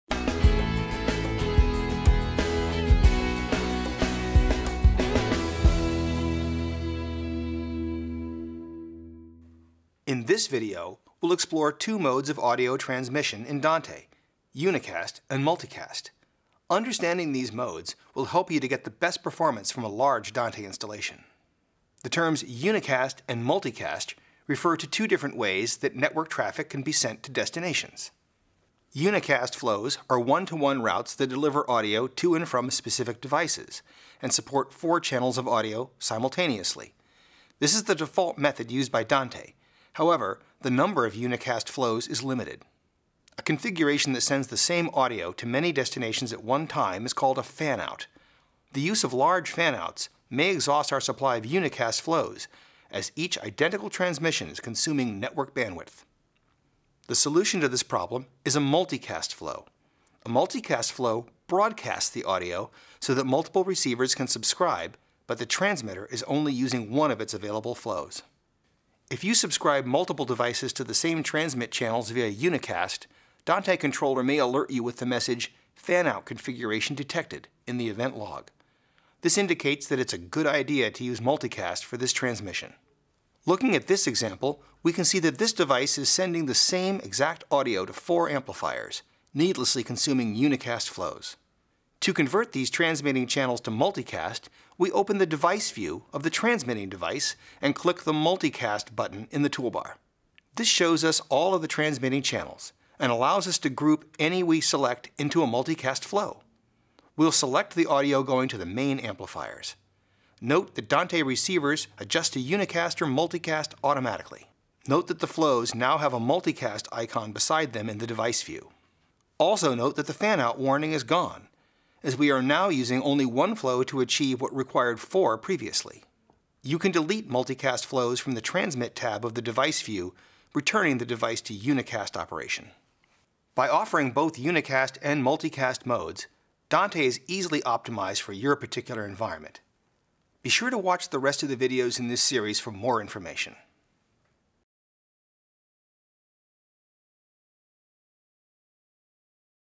Getting Started with Dante: 7. Unicast and Multicast Transport
7th in our series for Dante audio networking beginners:
 - Understanding Unicast and Multicast on IP networks
 - Best practices for use of Multicast
 - Configuring Multicast with Dante Controller